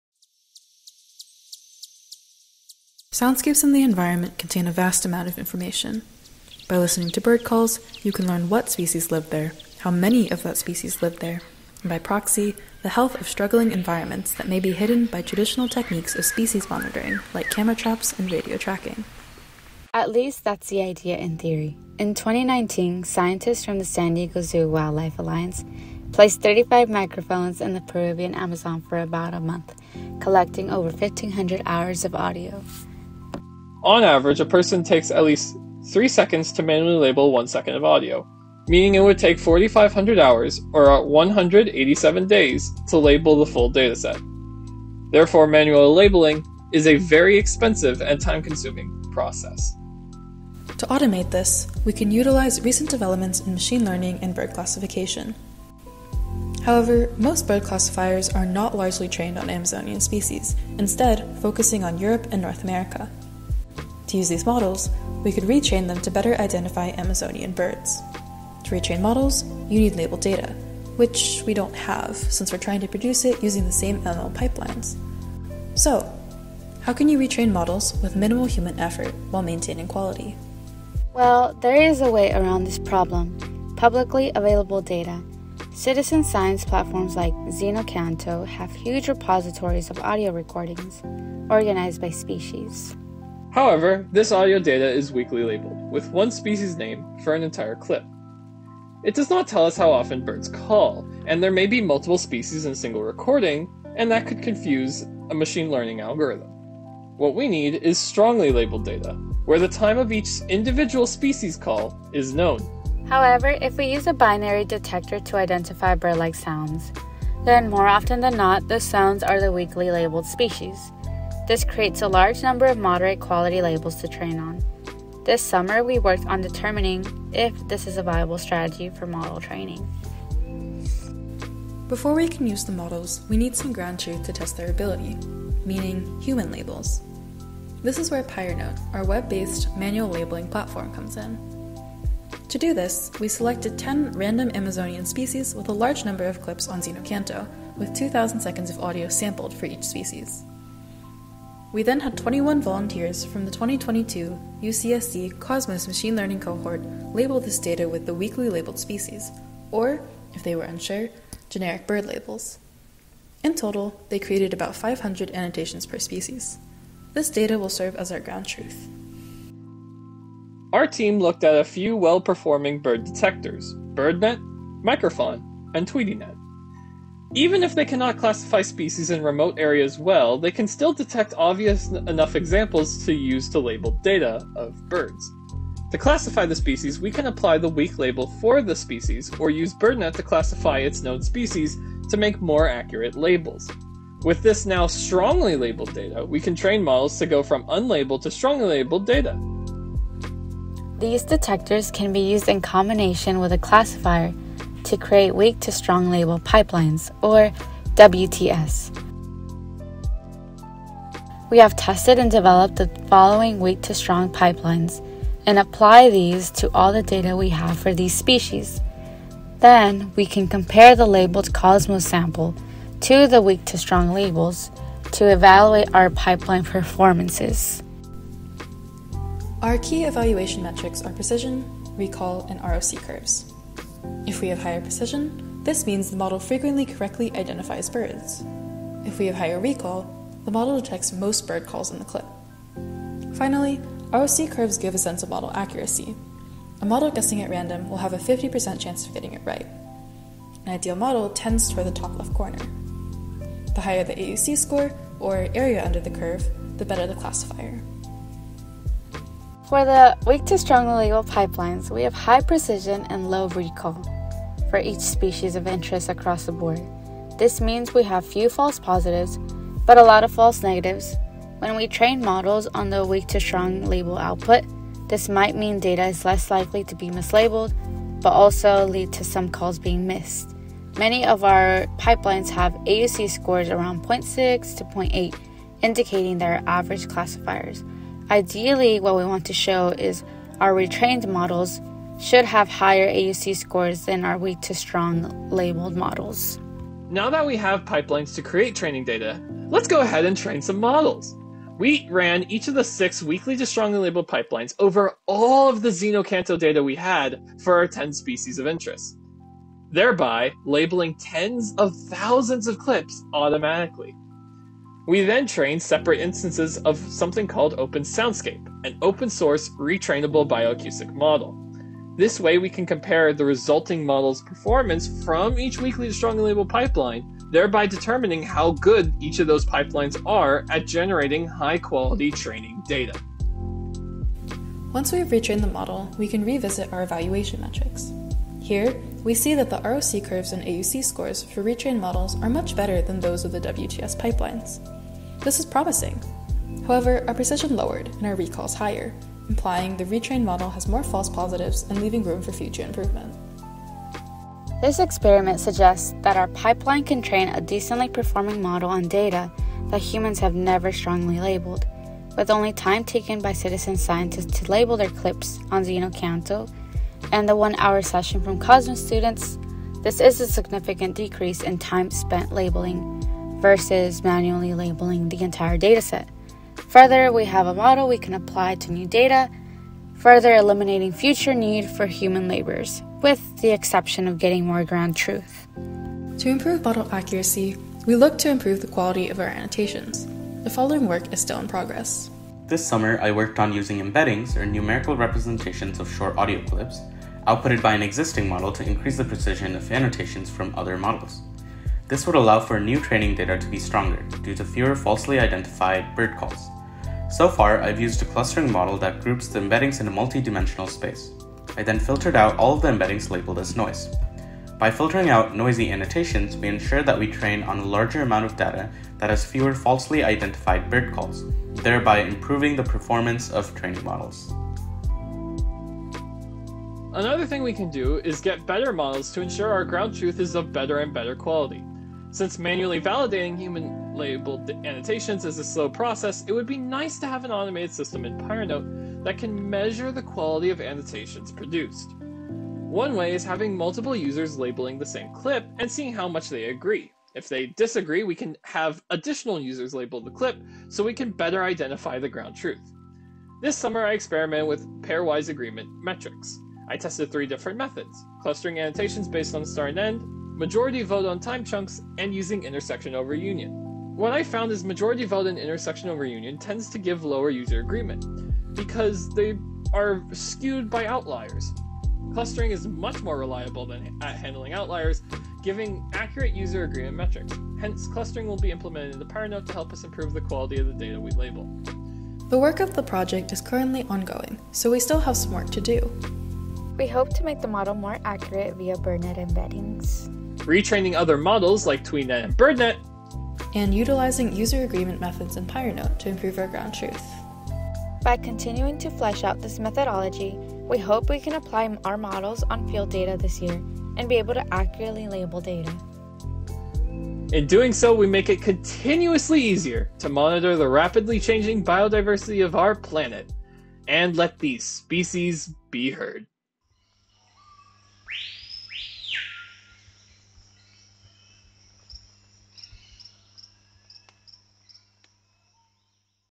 Automated Acoustics Species Id: Automated Training Data
Environment soundscapes hold rich information for measuring the biodiversity of many small, noisy species missed by traditional monitoring techniques. However, the effort needed to manually label thousands of hours of data at a species level for bio-analysis makes this infeasible. One could automate the labeling process with machine learning, but training a well-performing classifier requires labeled data. This summer, undergraduate students in the Automated Acoustics Species Identification Team in the 2022 E4E REU at the University of California, San Diego experimented with using pre-existing binary bird detectors to convert weakly labeled data (where a species is known to be present) into strongly labeled data (where the exact time of vocalizations is known) to train CNN/RNN hybrids. This proof of concept shows these “weakly to strongly labeled” pipelines can produce training data with few false positives and models with decent performance.